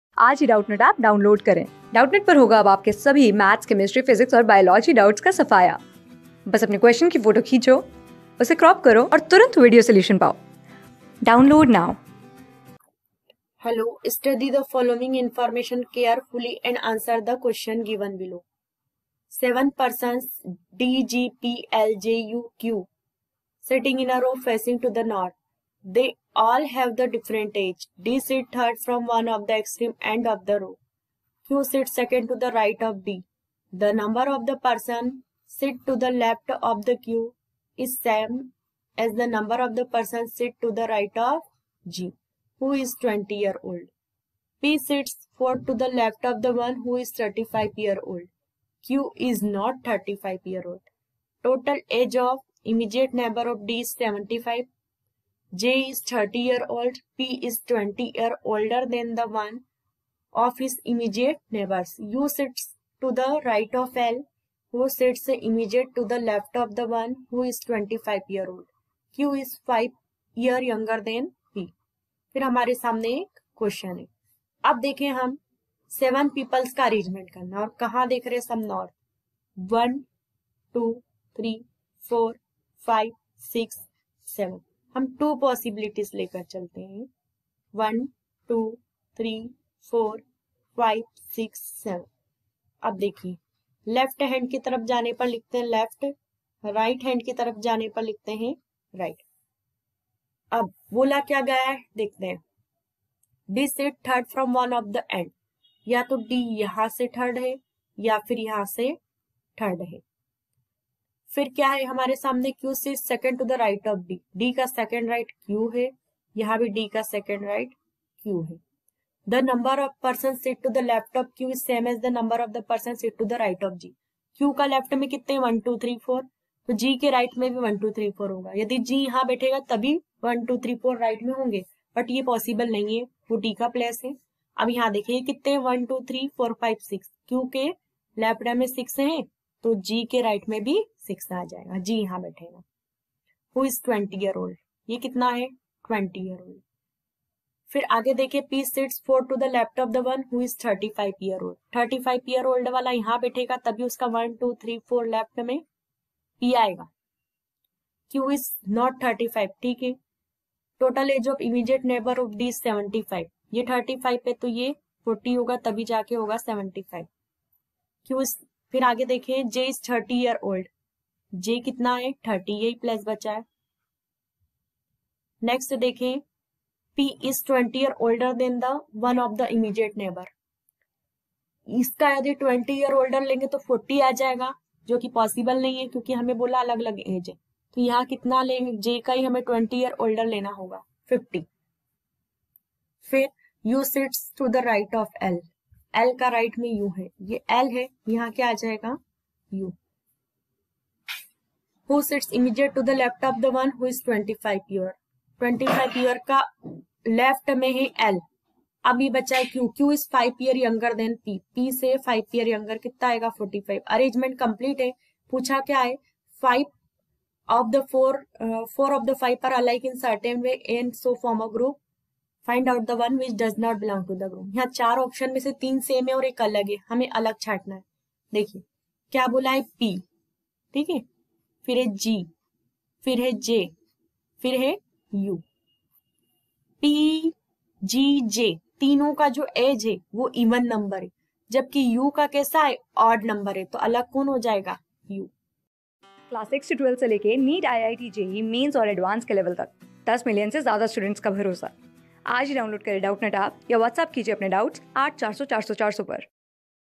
Study the following information carefully and answer the question given below Seven persons D, G...
Study the following information carefully and answer the question given below Seven persons D, G, P, L, J, U and Q are sitting in a row facing to the north. They all have of different ages. D sits 3rd from one of the extreme ends of the row. Q sits 2^(nd) to the right of D. The number of persons sit to the left of Q is same as the number of persons sit to the right of G, who is 20 years old. P sits 4^(th) to the left of the one who is 35 years old. Q is not 35 years old. Total age of immediate neighbours of D is 75 years. J is 30 years old. P is 20 year older than one of his immediate neighbours. U sits to the right of L, who sits immediate to the left of the one who is 25 years old. Q is 5 year younger than P. Four of the following five are alike in a certain way and so form a group. Find the one who does not belong to that group?
Class: 14
Subject: REASONING
Chapter: LINEAR SEATING ARRANGEMENT
Board:GOVT

👉 Have Any Query? Ask Us.